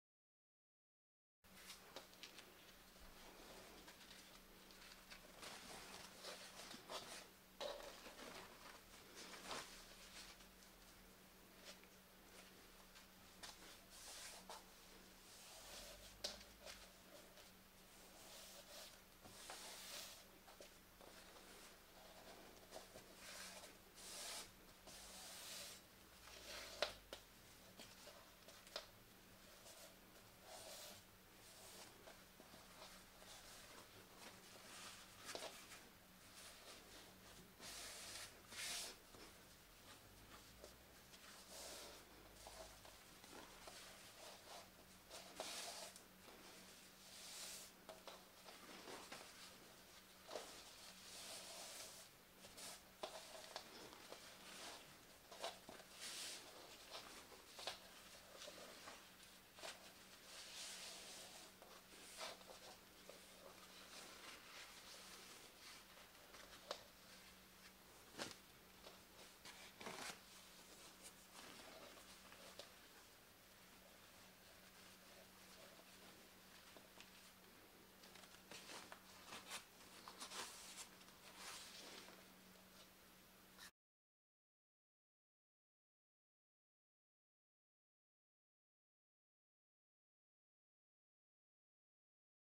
Servietten falten: 'Herbstblatt' für Herbst, Geburtstag & Weihnachten [W+]
Ein schönes Hersbtblatt für eine tolle Dekoration im Herbst, am Geburtstag und Weihnachten einfach und schnell basteln mit Wissen einfach und schnell [W+]